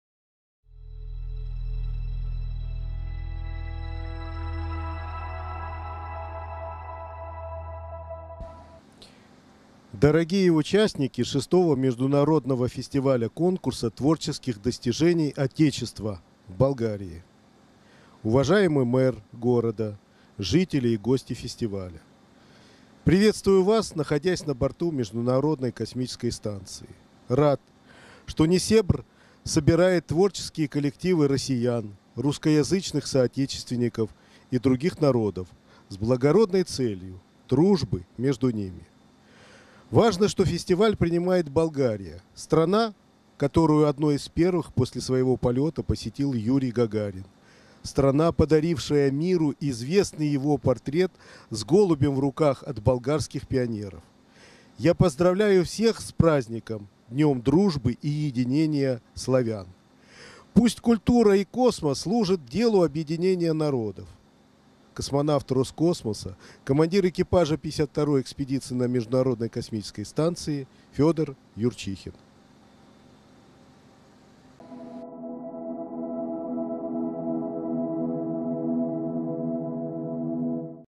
Космонавт Фёдор Юрчихин с борта МКС фестивалю "Отечество" в Болгарии
25 июня в День единения славян в болгарском городе Несебр на VI международном фестивале-конкурсе творческих достижений "Отечество" космонавт Роскосмоса Фёдор Юрчихин с борта МКС приветствовал его участников